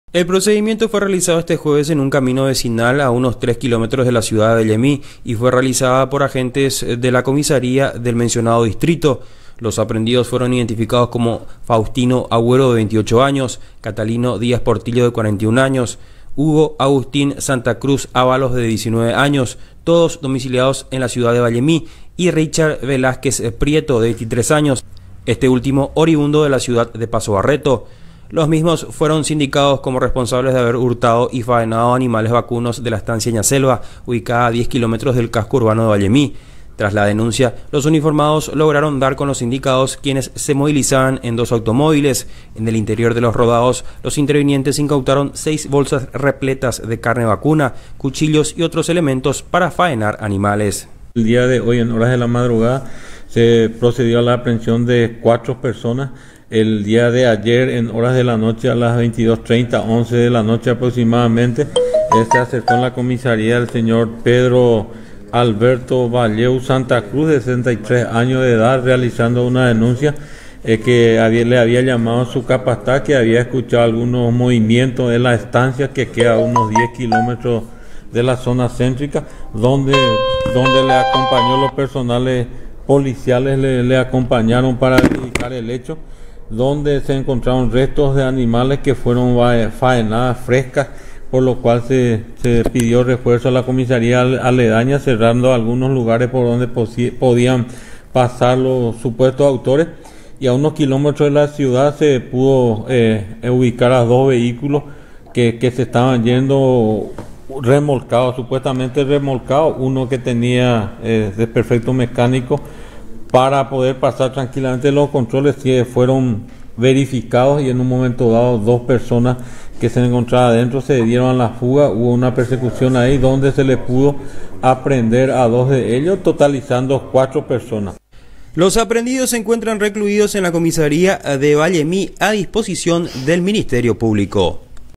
Caen 4 presuntos abigeos en Vallemí
Incautaron 2 vehículos repletos de carne vacuna